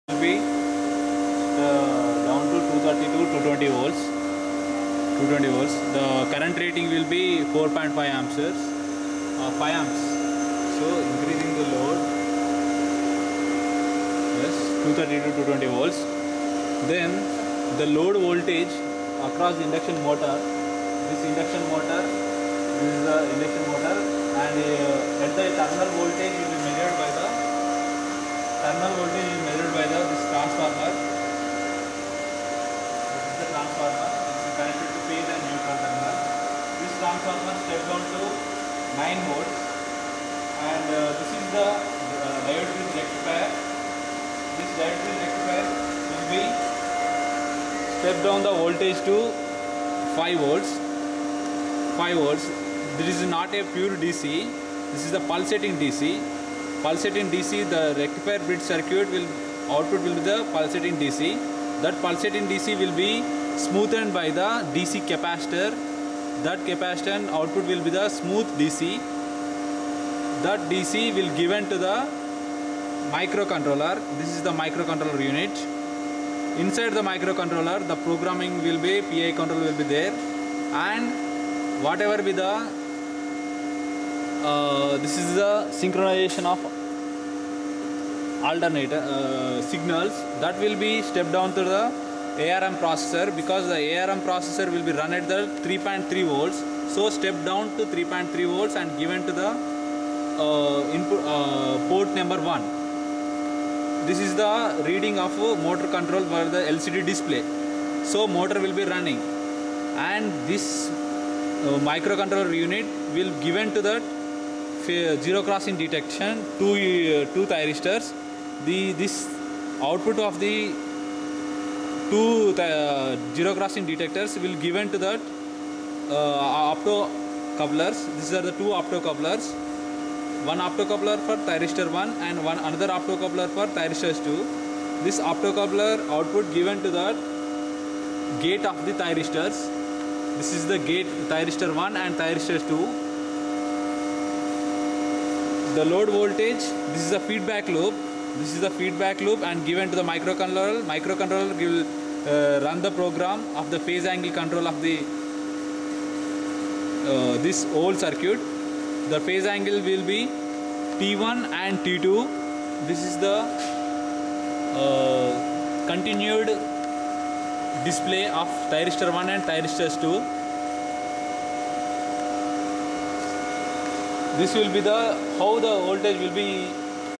STATIC VAR COMPENSATOR (SVC)
Flexible A.C. transmission System (FACTS) promottes the use of static controllers to enhance the controllability and increase the power transfer capability. Providing reactive shunt compensation with shunt-connected capacitors and reactors is a well established technique to get a better voltage profile in a poer system.
      i will design and fabrication of 1KVAr SVC using Micro-controller with different loads and observe the improvement of voltage as-well as reactive power compensation.